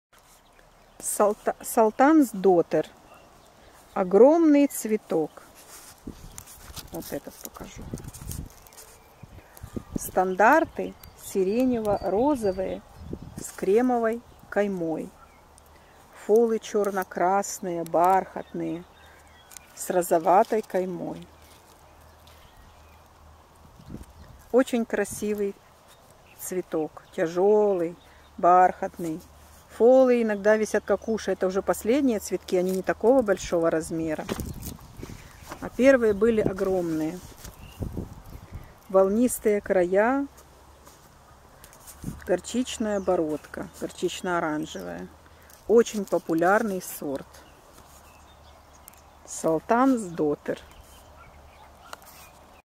Салтанс Дотер высокий ирис
Салтанс Дотер
Sultan's Daughter. Blyth '1995, ML, 90. S-сиренево- розовые с кремовой каймой, F-черно-красные с розоватой каймой.Тяжелый бархатный гофрированный, огромный!
Роскошный, бархатный, огромный.